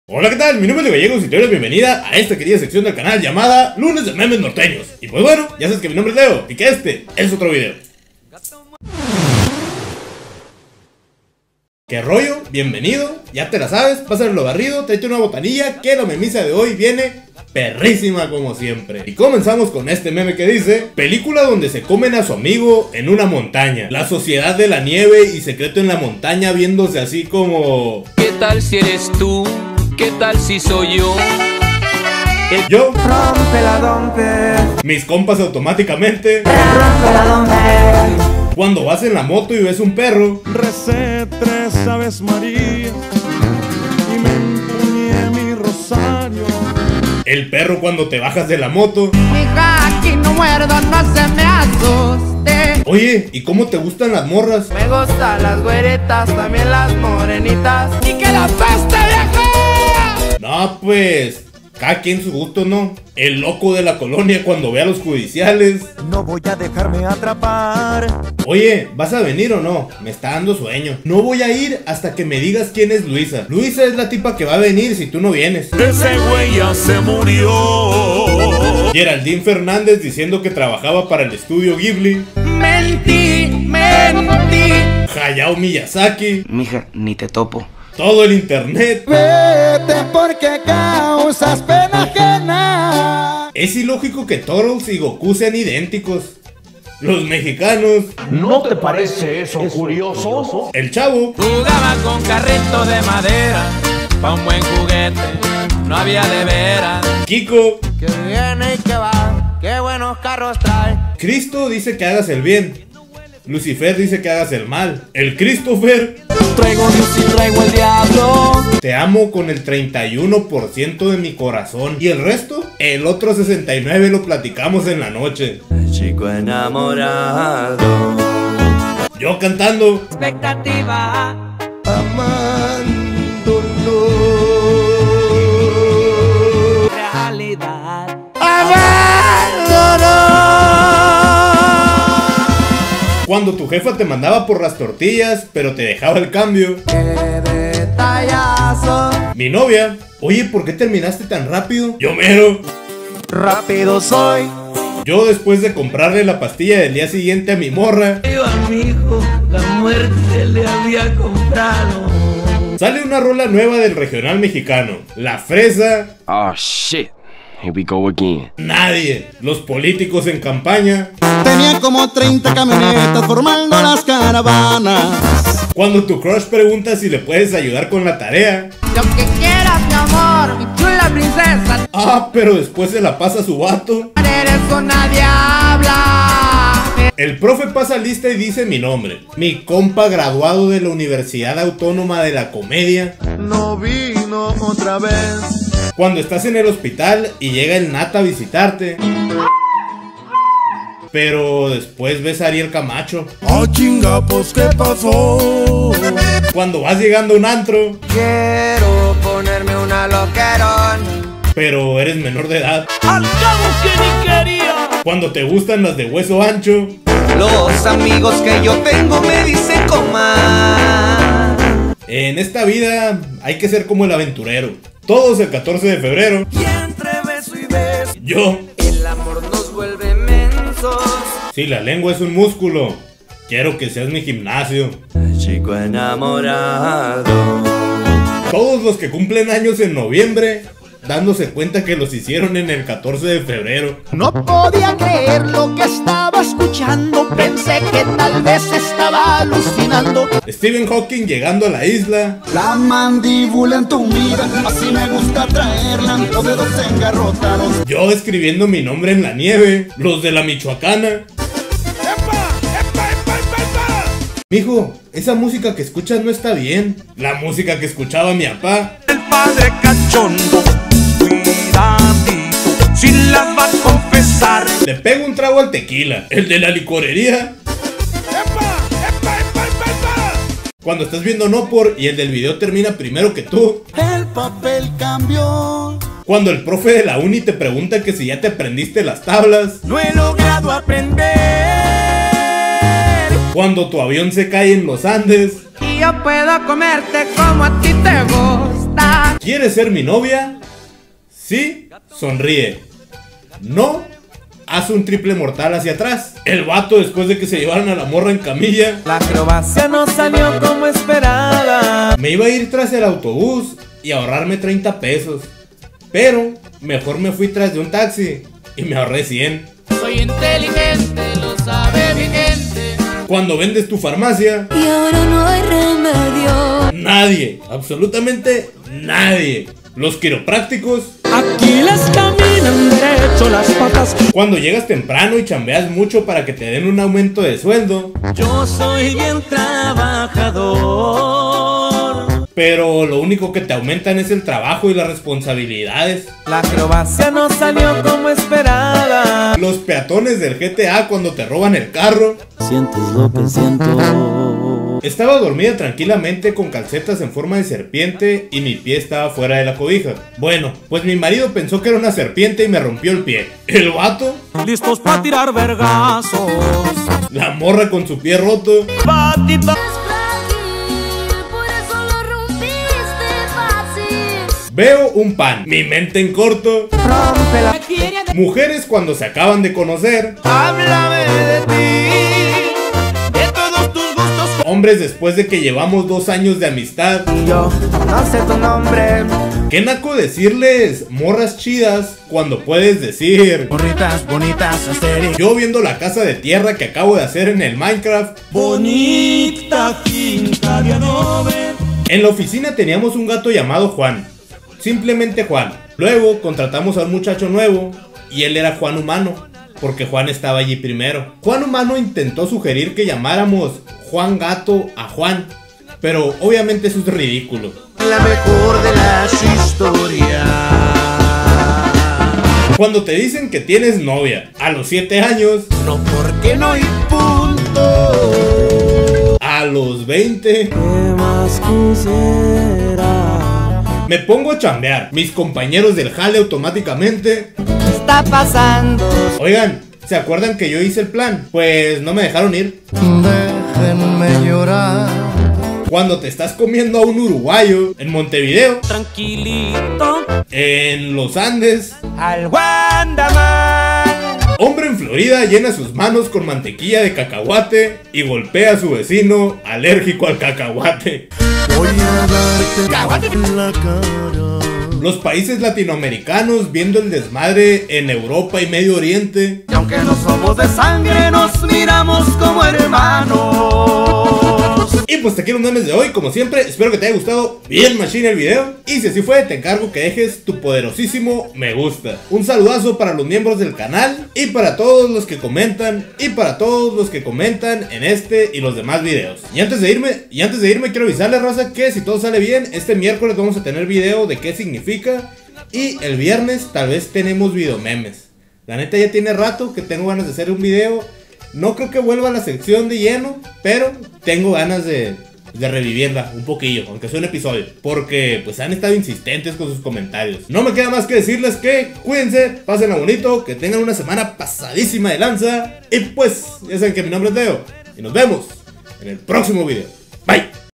Lunes de MEMES NORTEÑOS - Ep 321
Los mejores memes del mundo del Regional Mexicano, con memes sobre corridos de antaño, corridos tumbados y canciones norteñas en general.